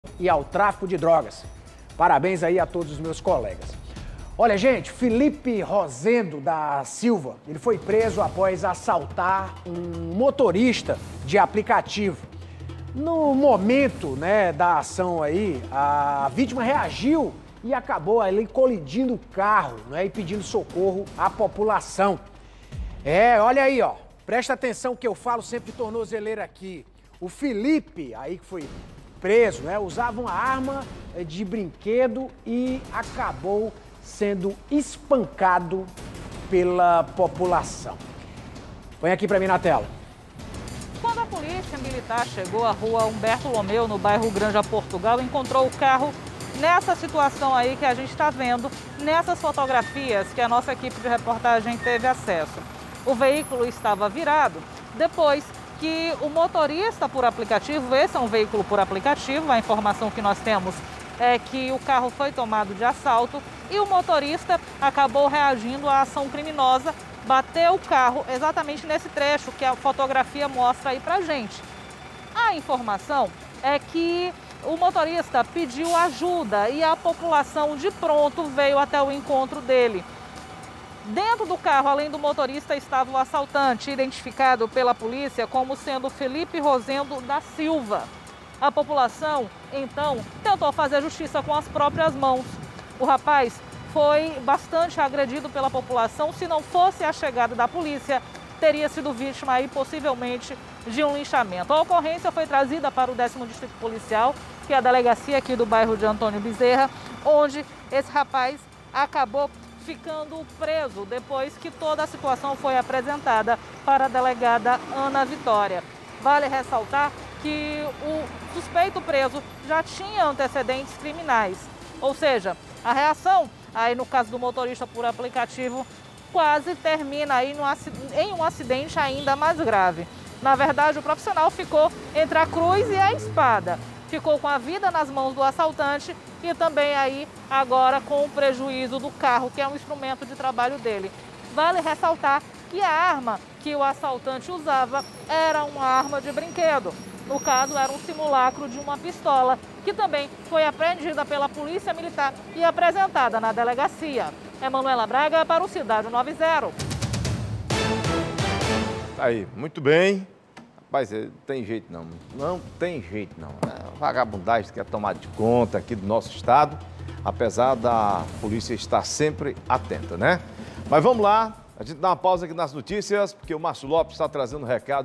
Bandido com arma de brinquedo apanha da população após assaltar motorista de aplicativo
Felipe Rosendo da Silva foi preso após assaltar um motorista de aplicativo. No momento da ação, a vítima reagiu, colidiu o carro e pediu socorro a população. Felipe usava uma arma de brinquedo e acabou espancado pela população.